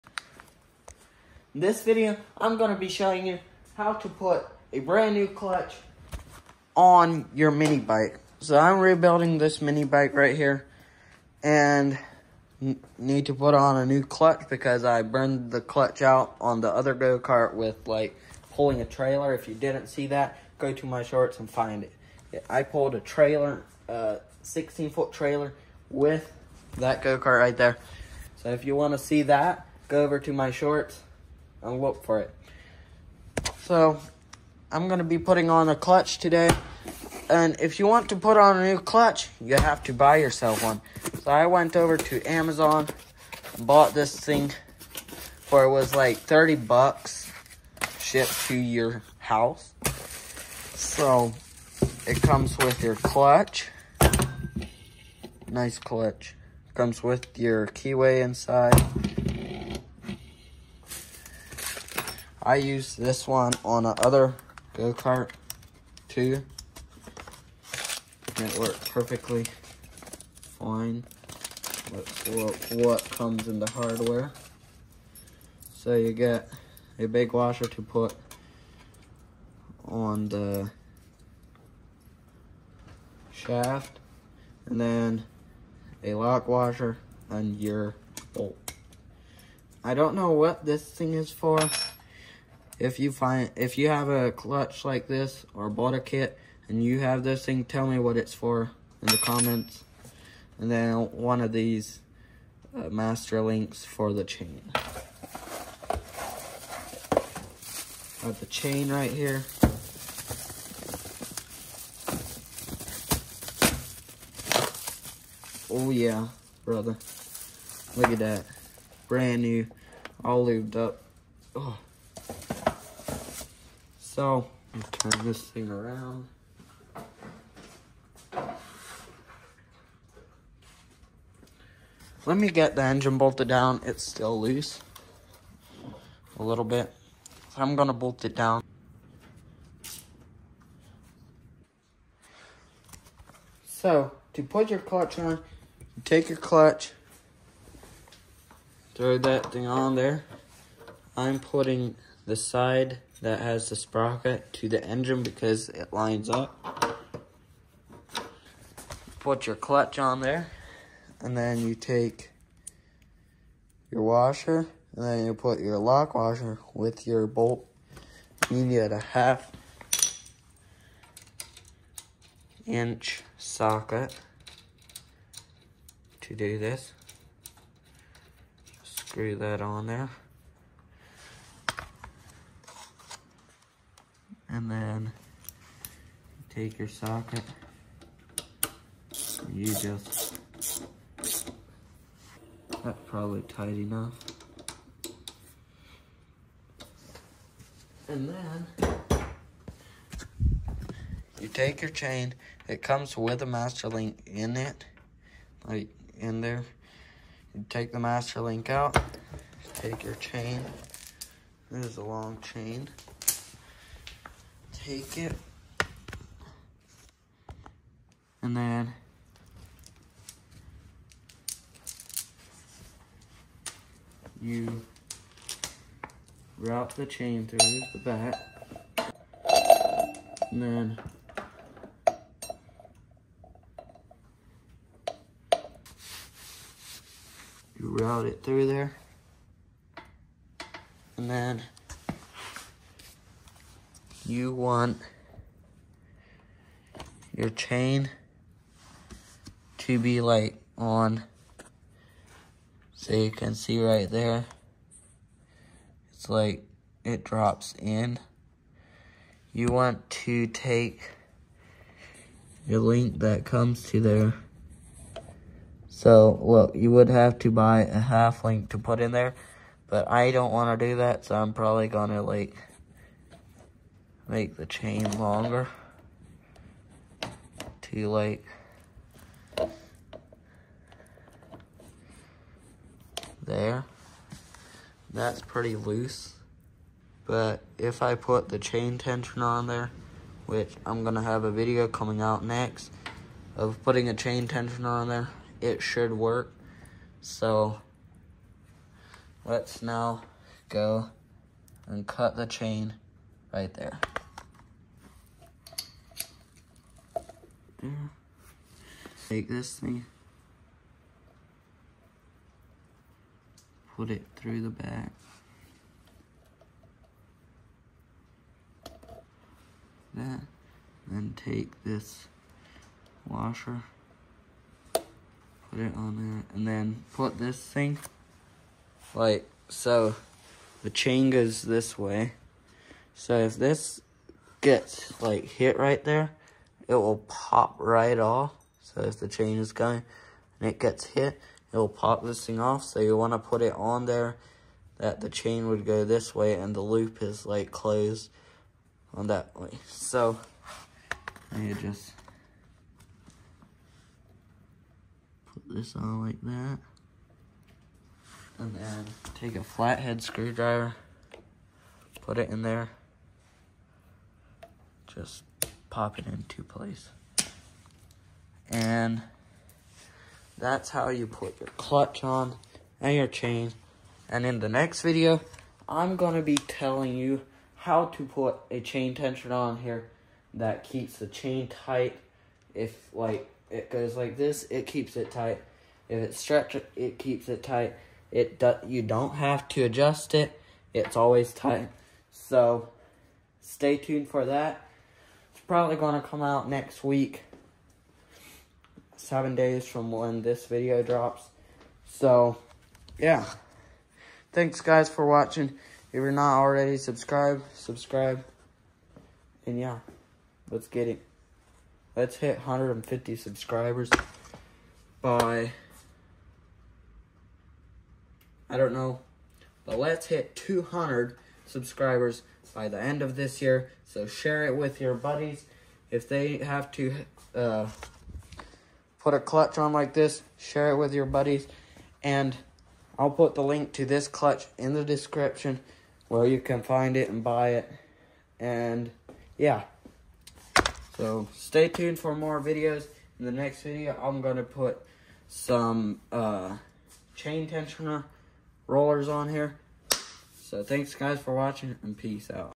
How to put a clutch and chain on your mini bike .
Clutch: KIKITE Go Kart Clutch 3/4" Bore 10 Tooth For 40/41/420 Chain, Minibike Chain Centrifugal Clutch 10T Fit 6.5HP